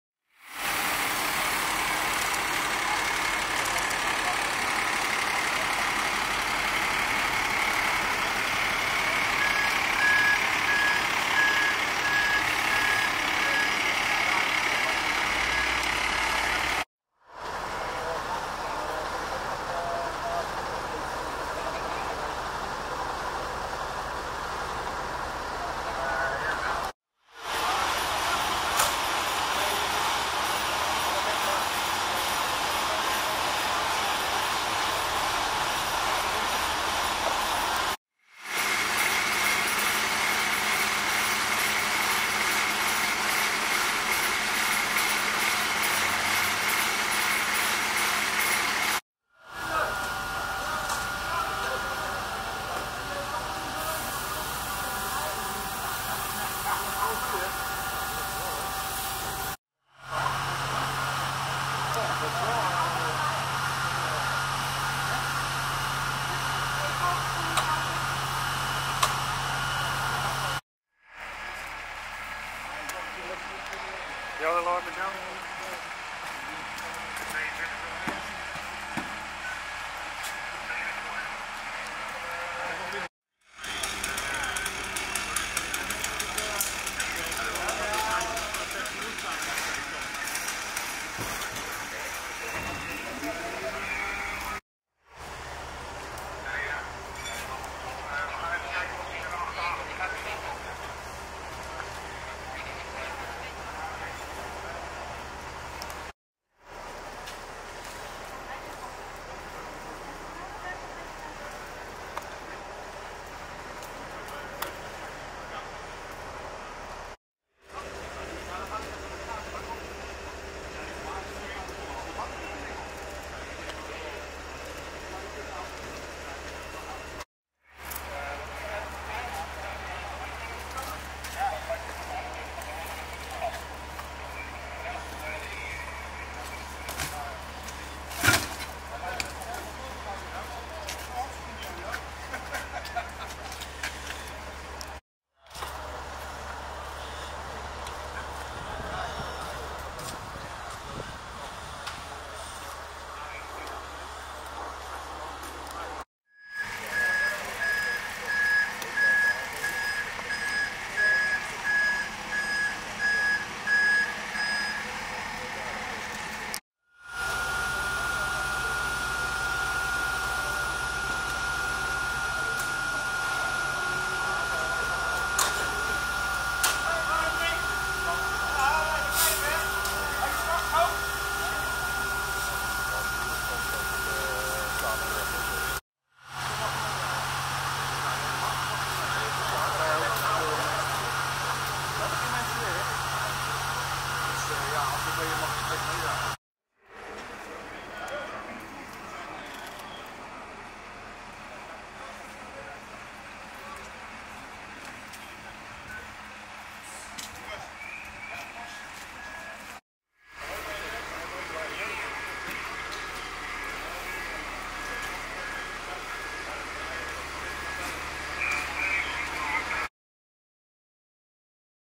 Brand met veel schade in timmerwerkplaats aan de Stadionweg in Rotterdam
Woensdagavond 25 september rond 18.05 uur is brand uitgebroken in een timmerwerkplaats aan de Stadionweg in Rotterdam.

De brandweer schaalde op naar middel brand, waardoor meerdere bluswagens ter plaatse kwamen om het vuur te bestrijden. Gelukkig raakte niemand gewond tijdens het incident.

De brand veroorzaakte echter aanzienlijke schade aan de werkplaats. De oorzaak van de brand wordt nog onderzocht.